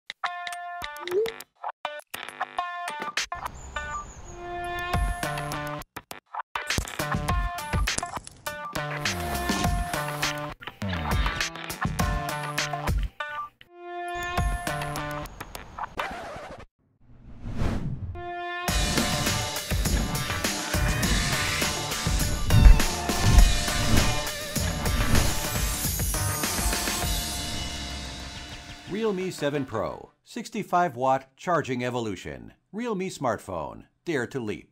Charge Faster than MakeUp | realme 7 Pro charge 3 mins get 13%
Don’t let your boyfriend/girlfriend wait for you for more than 3 minutes!

Charge N Go.